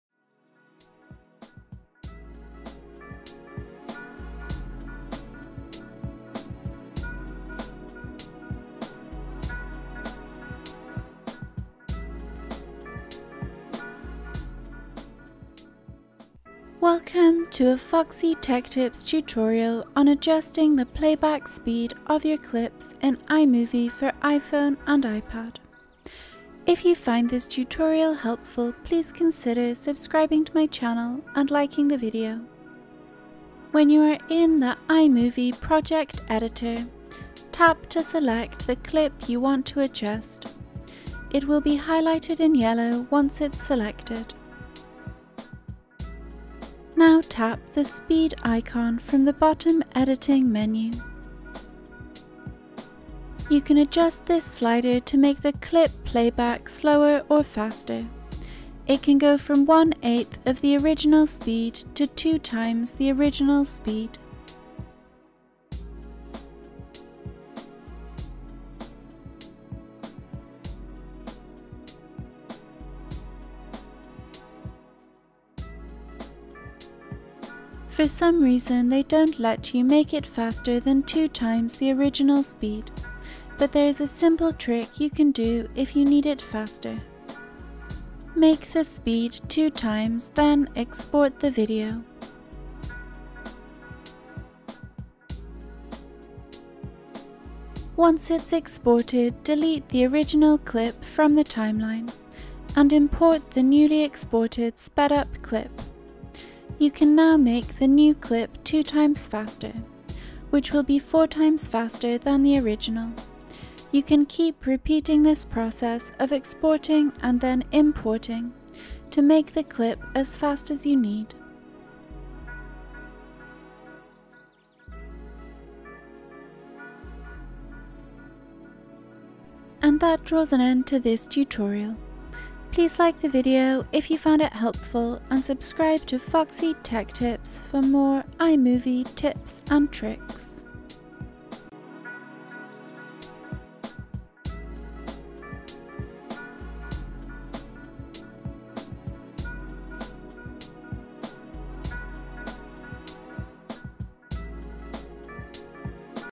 How To Speed Up & Slow Down Clips in iMovie (iPhone & iPad)
Easy to follow tutorial on adjusting the speed of your video clips in iMovie for iPhone & iPad! Learn how to speed up your clips and how to slow them down in iMovie. By Default, iMovie only allows you to speed it up by 2x, but I'll show you a trick that allows you to speed up your clips an unlimited amount.

Steps:

- When you are in the iMovie project editor, tap to select the clip you want to adjust, It will be highlighted in yellow once its selected.

- Now tap the speed icon from the bottom editing menu.

- You can adjust this slider to make the clip playback slower or faster. It can go from 1/8th of the original speed to 2x the original speed.

- For some reason, they don’t let you make it any faster than 2x the original speed, but there is a simple trick you can do if you need it faster.

- Make the speed 2x, then export the video.

- Once it’s exported, delete the original clip from the timeline and import the newly exported sped up clip, you can now make the new clip 2x faster which will be 4x faster than the original. you can keep repeating this process of exporting and then importing to make the clip as fast as you need.